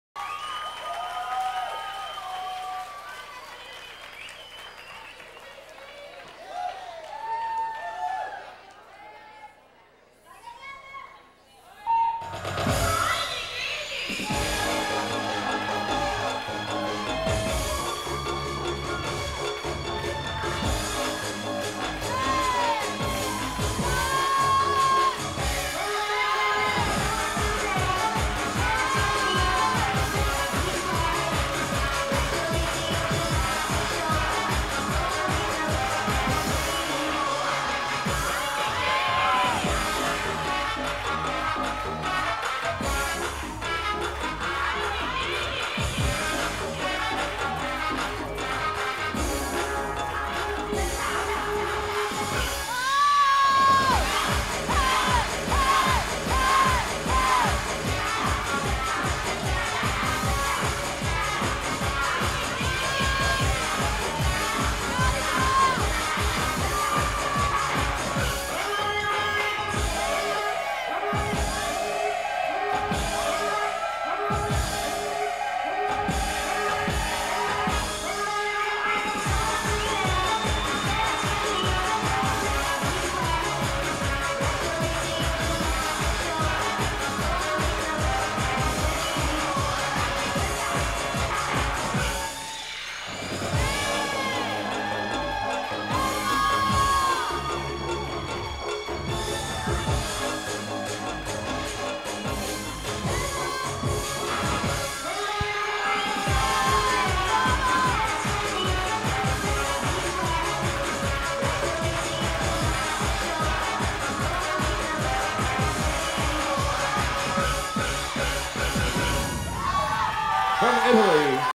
Arianna Ciucci (Italy) - 2000 World Youth Aerobic Championship Finals
Italy's Arianna Ciucci at 2000 World Youth Aerobic Championship
Placement: 2

The music in this video has been edited and remixed by Marco Manara Sound Factory
FB: The Sound Factory - Marco Manara Music

Choreography: Nicoletta Minestroni, Gigliola Carosi